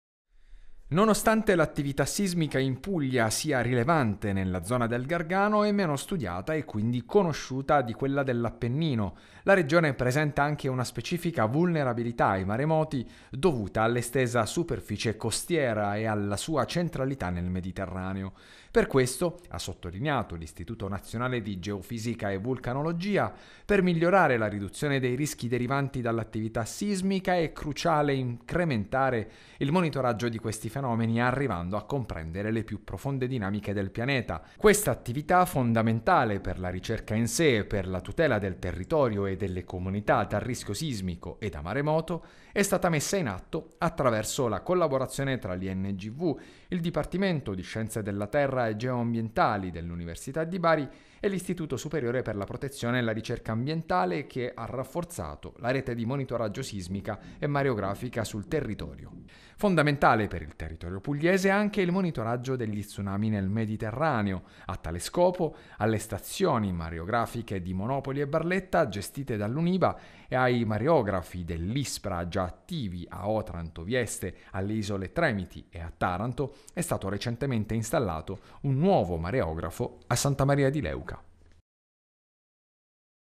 Puglia, Ingv: implementato il monitoraggio sismico e mareografico
L’Istituto nazionale di geofisica e vulcanologia ha implementato il monitoraggio sismico e mareografico